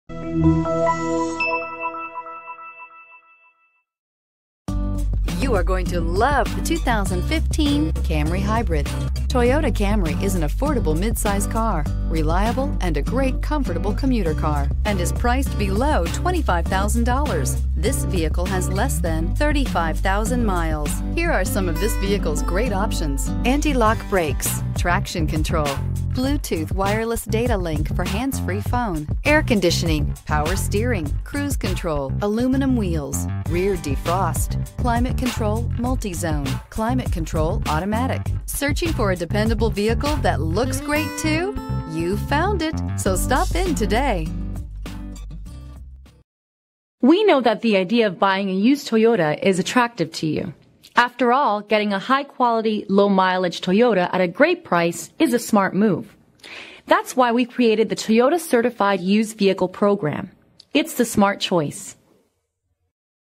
2015 Toyota Camry Hybrid SE Maplewood, St Paul, Minneapolis, Brooklyn Park, MN P17026
Maplewood Toyota
2873 Maplewood Dr

Maplewood Toyota
2873 Maplewood Drive

Pick up this Celestial Silver Metallic 2015 Toyota Camry Hybrid, available today at Maplewood Toyota. This could be the one you've been searching for.